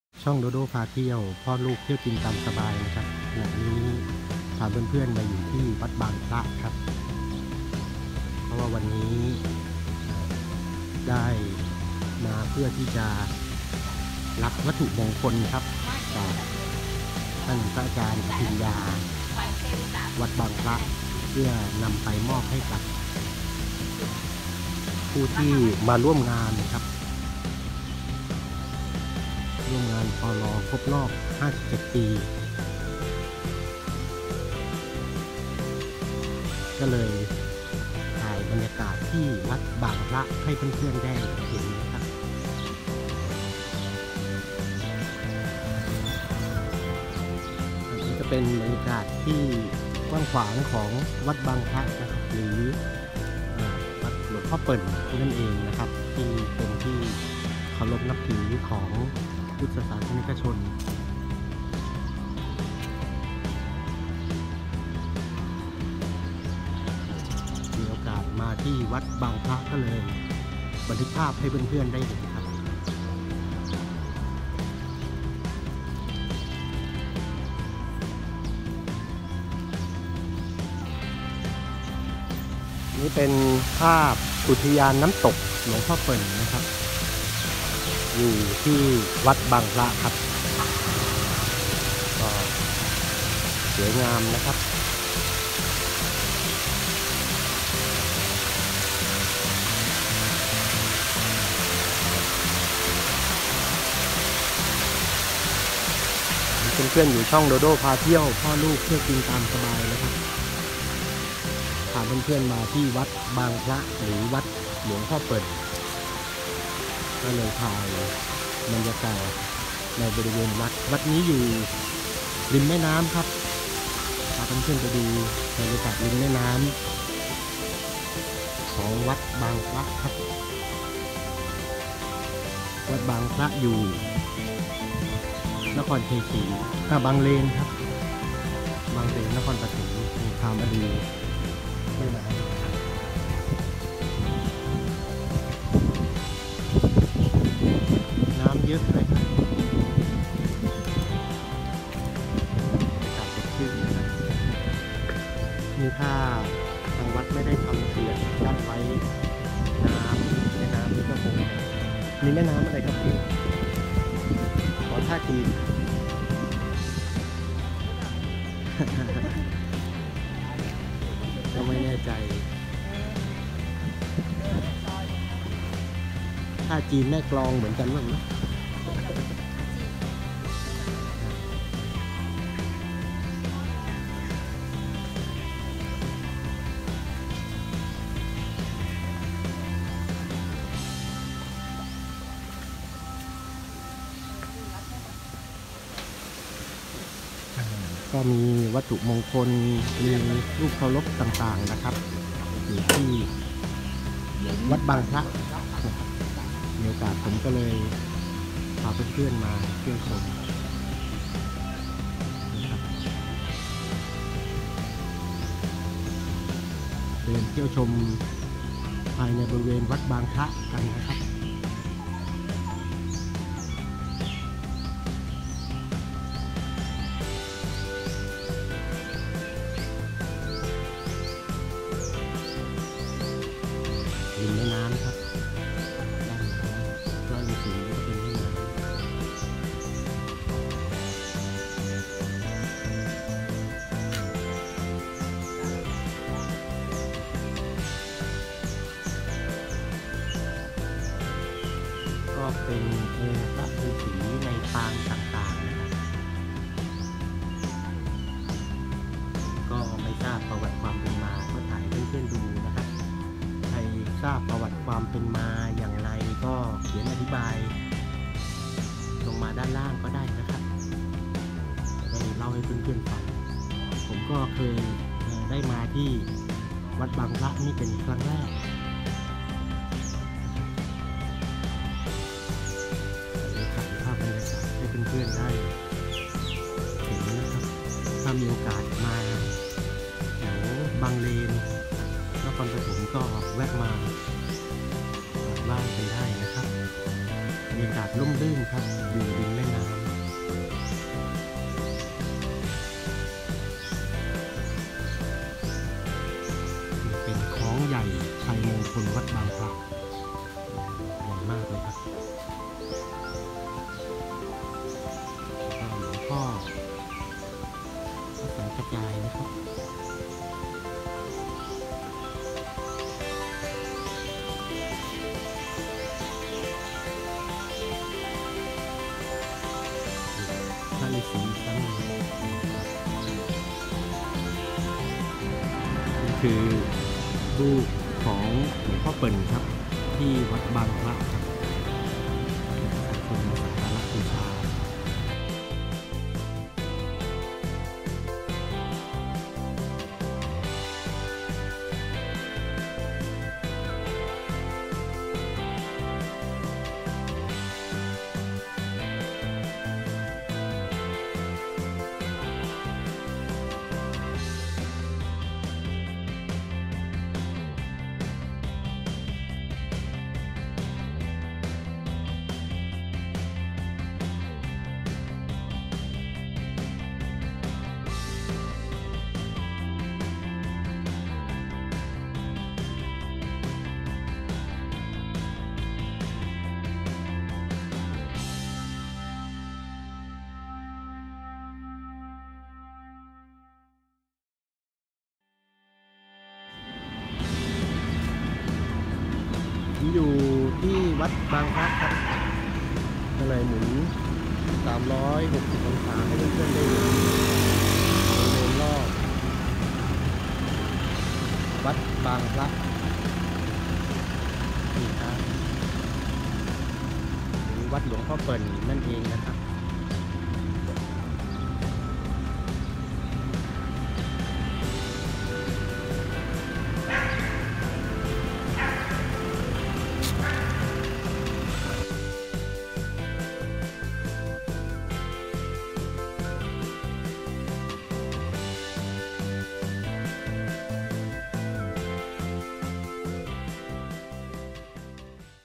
พาไปดู ฤษี ที่วัดบางพระ บางเลน นครปฐม (วัดหลวงพ่อเปิน) Take a look at the hermit
ไปรับวัตถุมงคล ที่วัดบางพระ ต.บางเลน จ.นครปฐม (วัดหลวงพ่อเปิน) พาไปดูรูปฤษี และกราบหลวงพ่อเปิน ครับ